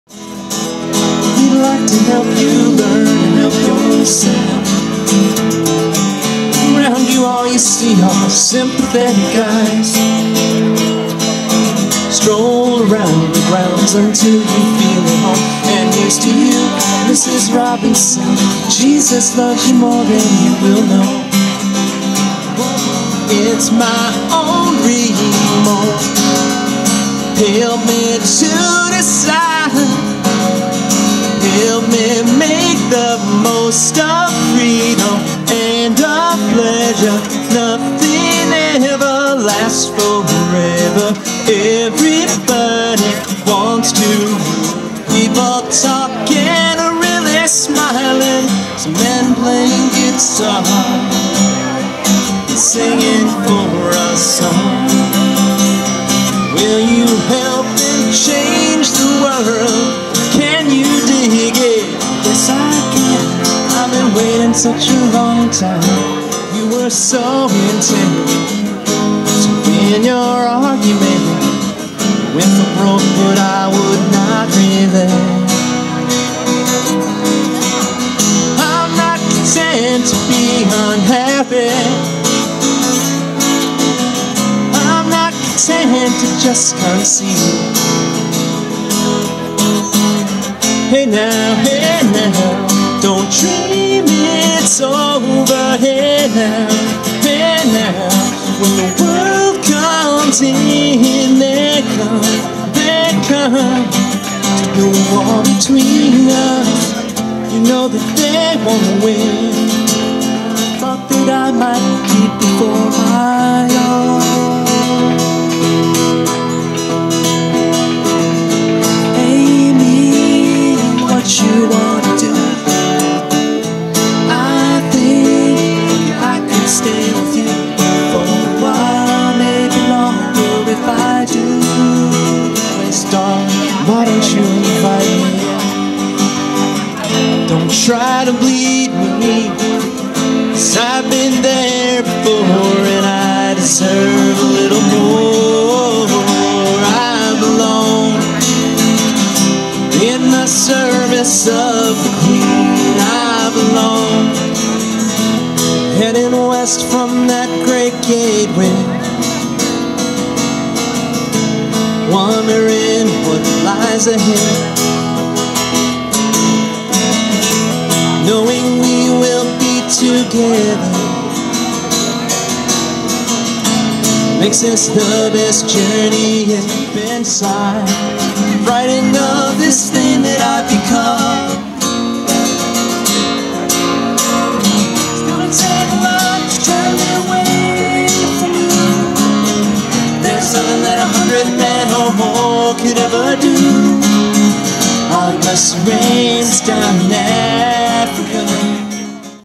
Street Level Doppler Promo Video (Sep. 2009)
Street Level Doppler Promo Video. Live acoustic performance (Sep. 2009)